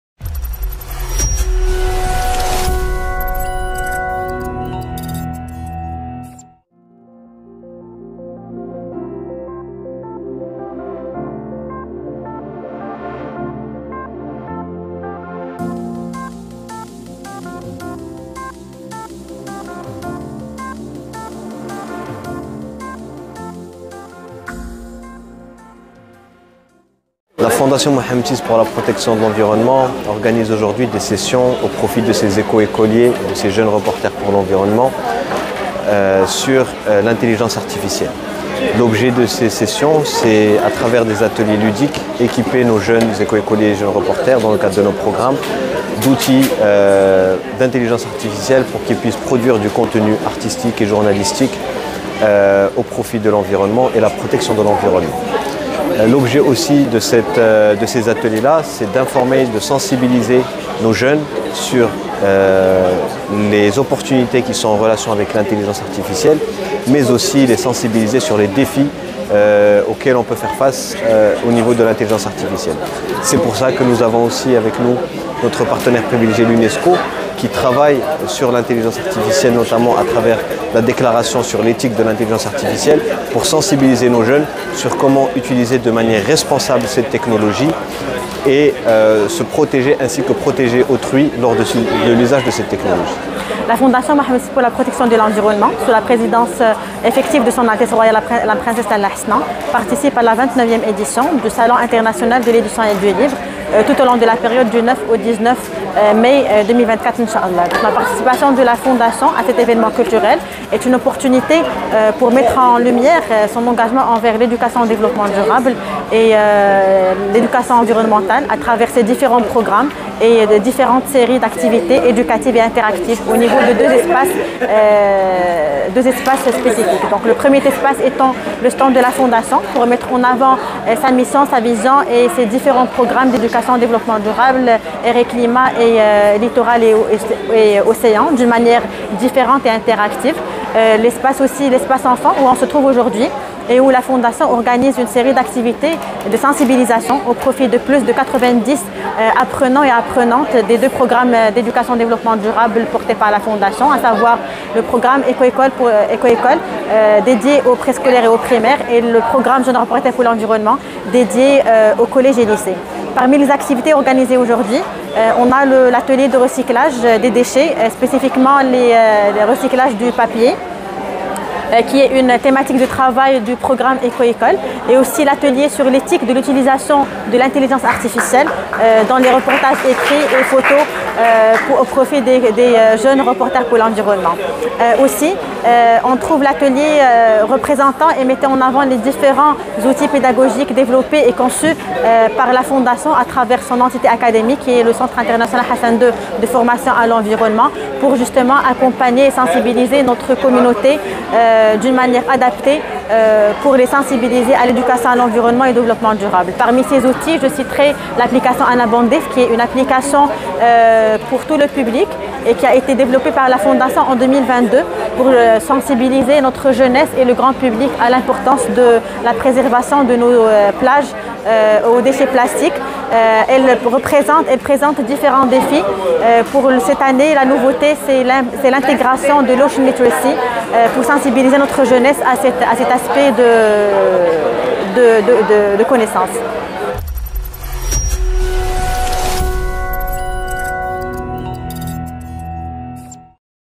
SIEL 2024: Ateliers de sensibilisation de la Fondation MVI pour la protection de l’environnement
SIEL 2024: Ateliers de sensibilisation de la Fondation Mohammed VI pour la protection de l’environnement au profit des éco-écoliers